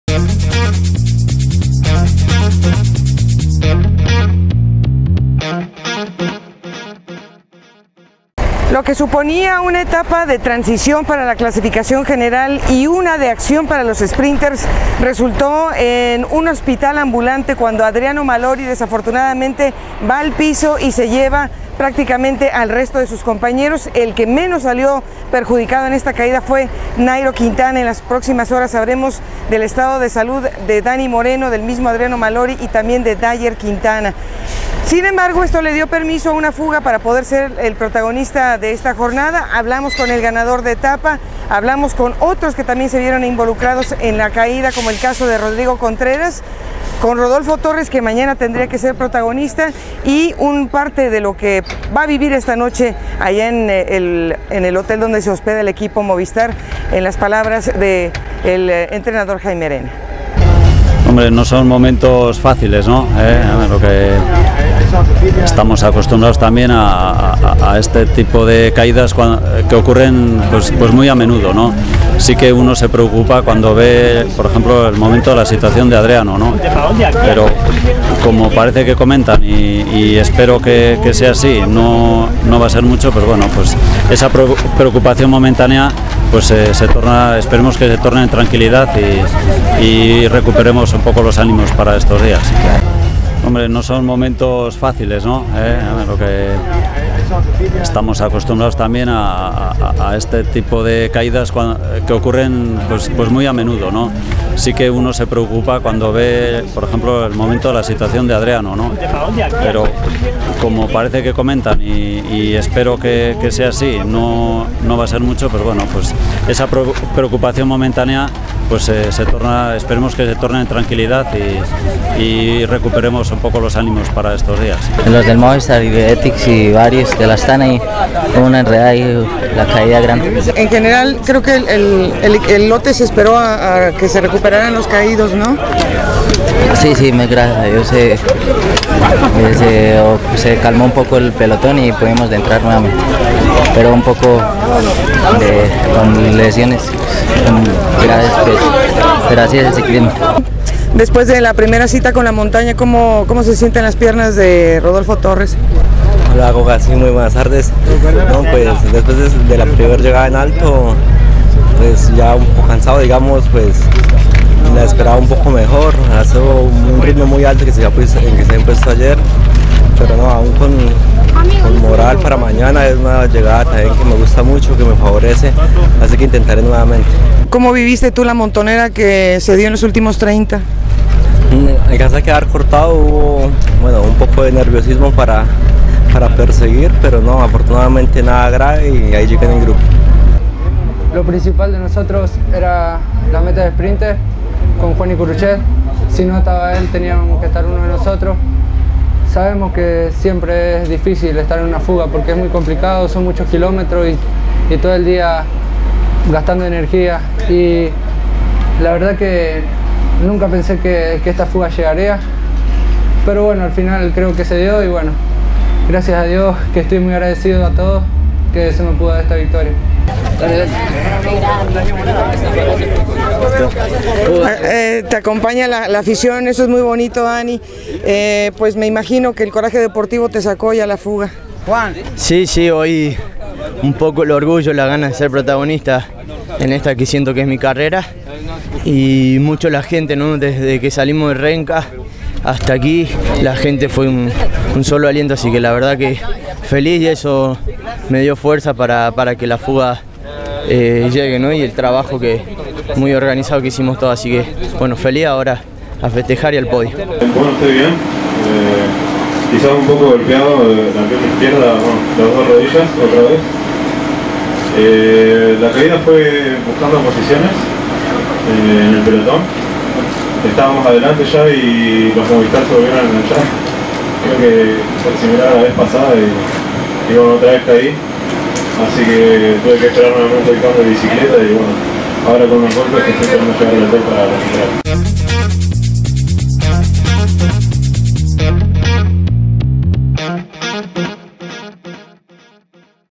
Reacciones tras la caida de la Etapa 5 y German Tivani (ARG) ganador en figa
Tras una montonera donde quedo afectado Adriano Malori la fuga coronó la etapa 5 del Tour de San Luis 2016. Entrevistas con
José Luis Jaimerena (DD Movistar)
Rodrigo Contreras (Sub23)
Rodolfo Torres (AND)
Dani Diáz (2º en la etapa)
Germán Tivani (Ganador etapa)
Eduardo Sepúlveda (Conservó el liderato)